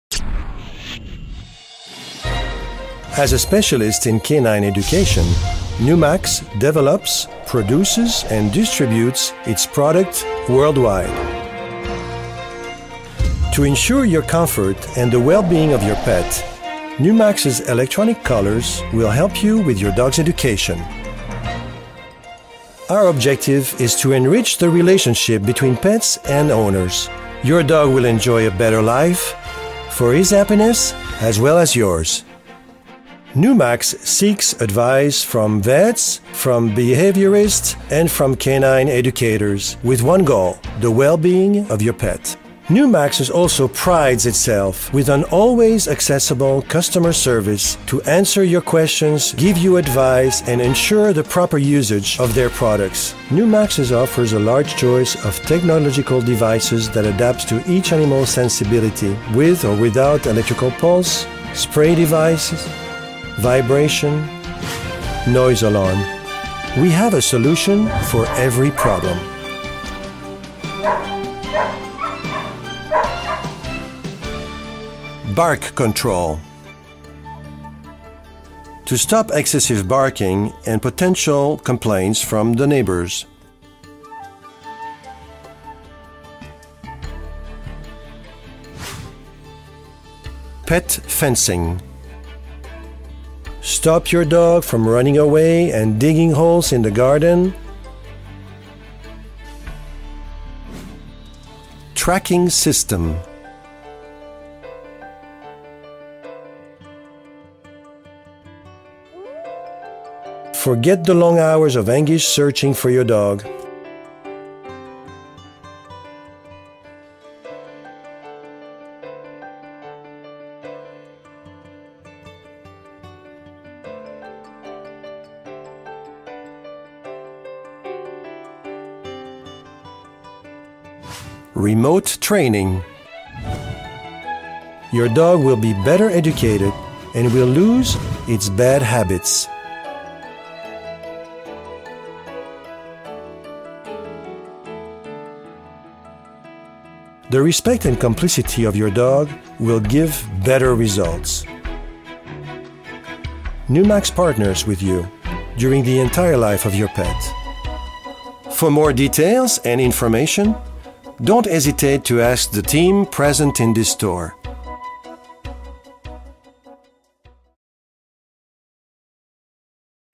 Num'Axes Produktvideo
Num'Axes vertreibt elektronisches Zubehör für Jagd- und Gebrauchshunde. Ein kleiner Animationsfilm stellt die verschiedenen Produkte vor.